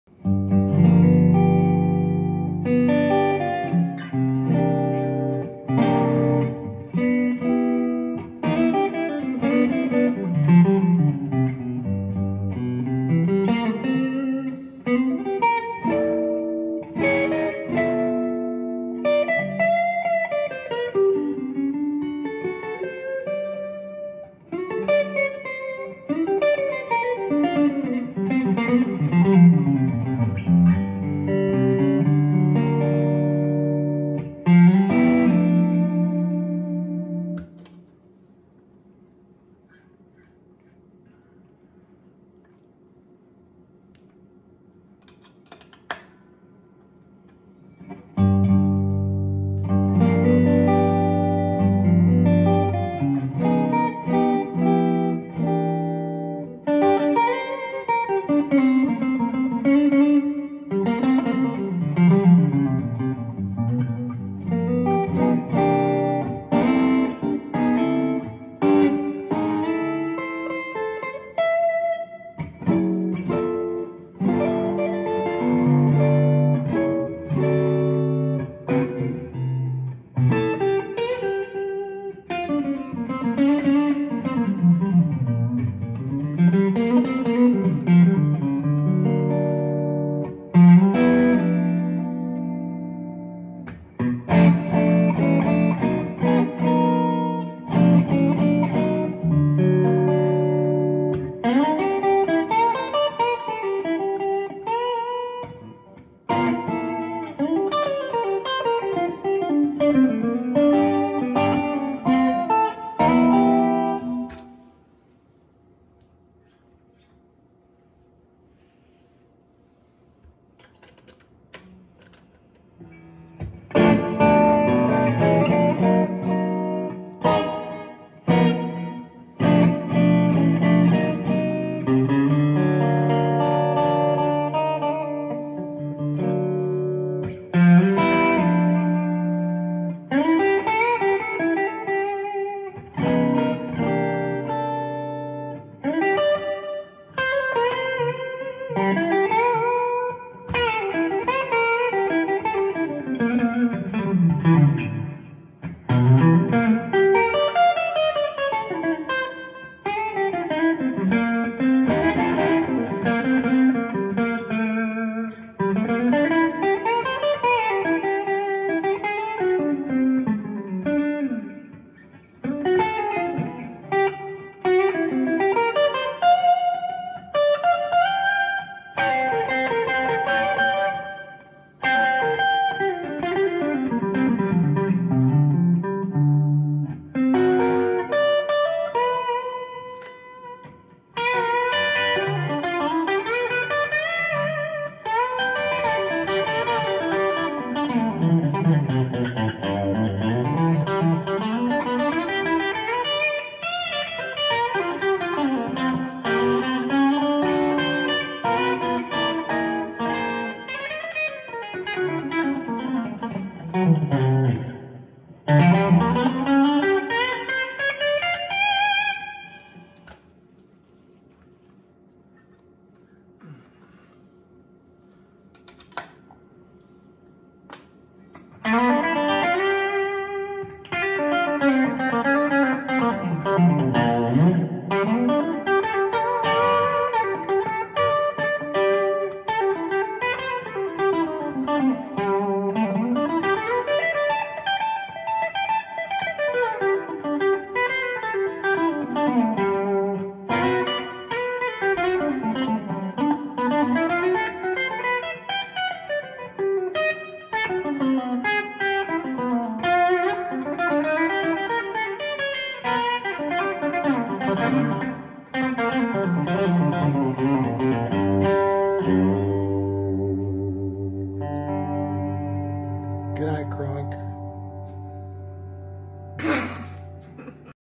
1956 Stratocaster Versus Parts-caster Ben Cloud
Original Feb 1956 Strat versus parts-caster with baked ash body, Fender Custom Shop neck, Bareknuckle Apache pickups.  The 1956 had just been restrung (our last set) so the comparison isn’t fair to the parts-caster.

Parts-caster built by steve “Doc” Hickerson, Tulsa OK